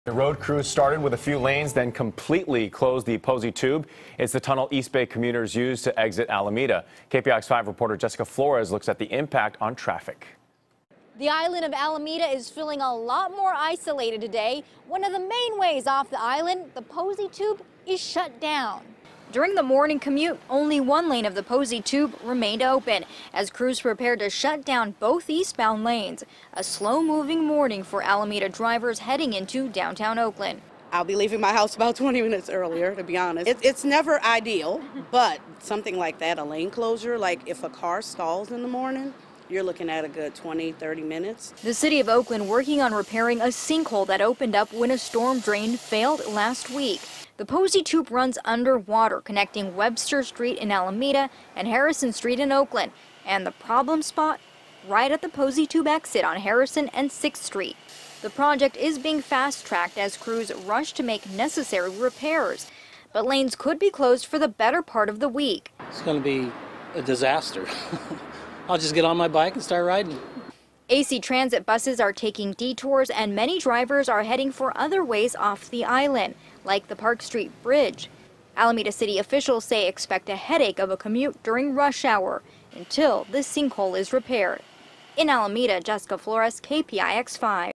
Sinkhole Repairs Shut Down Both Lanes Of Posey Tube
Jessica Flores reports on closure of eastbound lanes of Posey Tube connecting Alameda to Oakland for sinkhole repairs (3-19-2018)